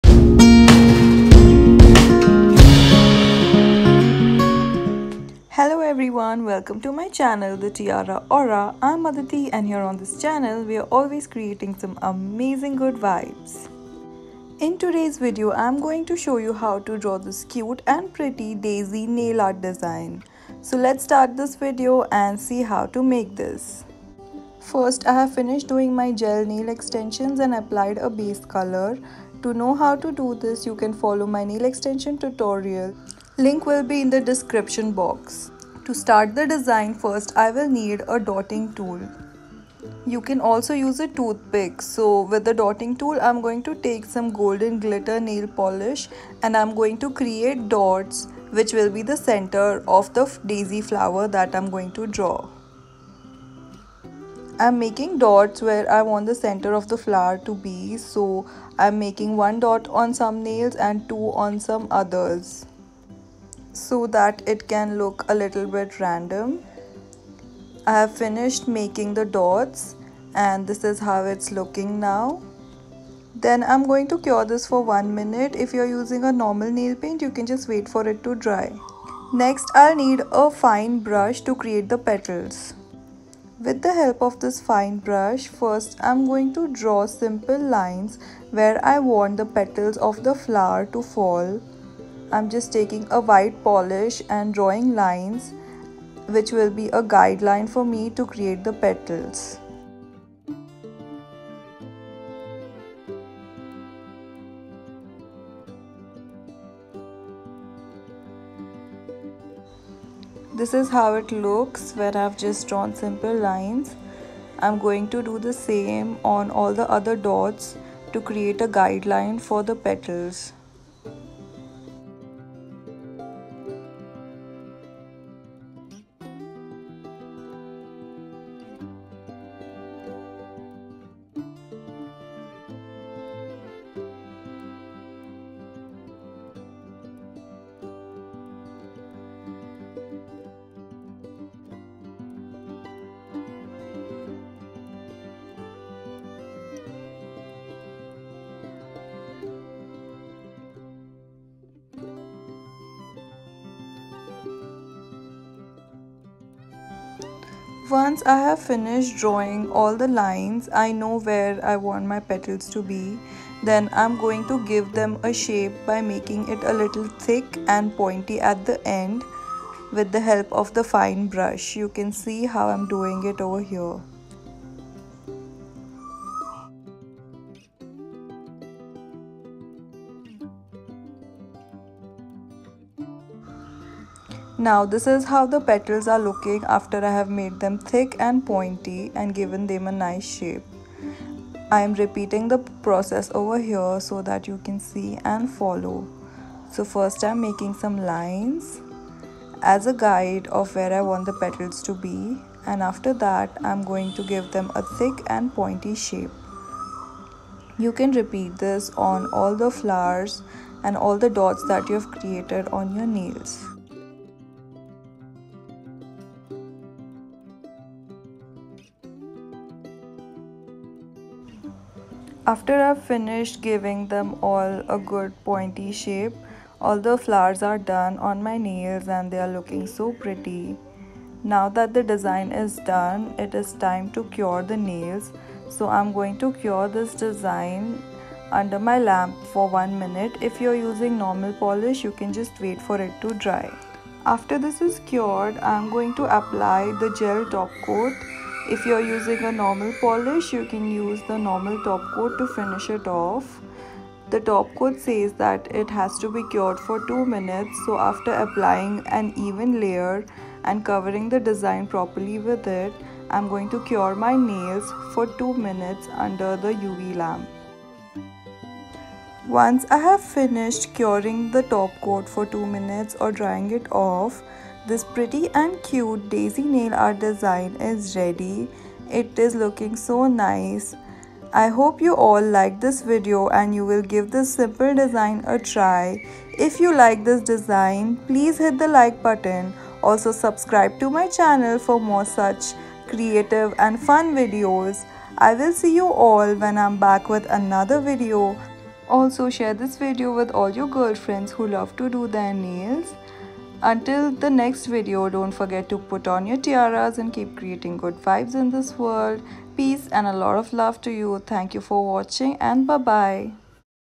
Easy cute Daisy nail art design🌼| Floral Nail Art Design | Summer nails
In this video I show you how to create simple Daisy floral nail art design perfect for summer style.